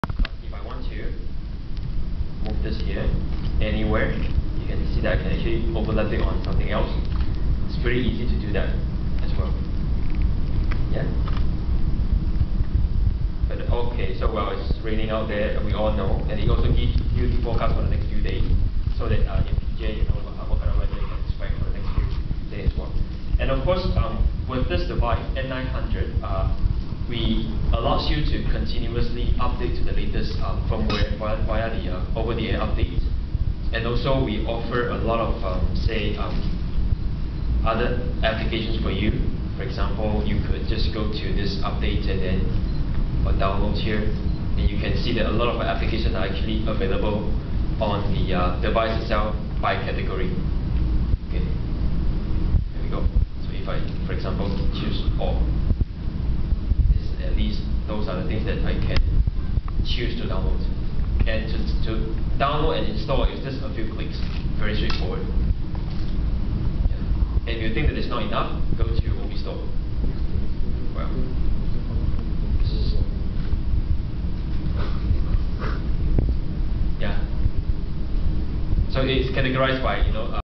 Nokia N900 Workshop at Royale Bintang Damansara Hotel, Malaysia
Glen Cha, Product Manager of Nokia Singapore/Malaysia & Brunei, is giving an in-depth demonstration of the Nokia N900. Here is part of the demonstration.

The Nokia N900 will go on sale at RRP RM2,280 in selected Nokia stores in Malaysia from 27 March 2010.

The Nokia N900 packs a powerful ARM Cortex-A8 processor, up to 1GB of application memory and OpenGL ES 2.0 graphics acceleration.

One of the advantages of Nokia N900 is its multitasking capability , allowing many applications to run simultaneously smoothly just like any PC.

The panoramic homescreen can be fully personalized with favorite shortcuts, widgets and applications.

Web browsing using Nokia N900 is outstanding as it features a high-resolution WVGA touch screen and fast internet connectivity with 10/2 HSPA and WLAN capabilities. Thanks to the browser powered by Mozilla technology, websites look the way they would, just like on any computer. Online videos and interactive applications are vivid with full Adobe Flash(TM) 9.4 and AJAX support.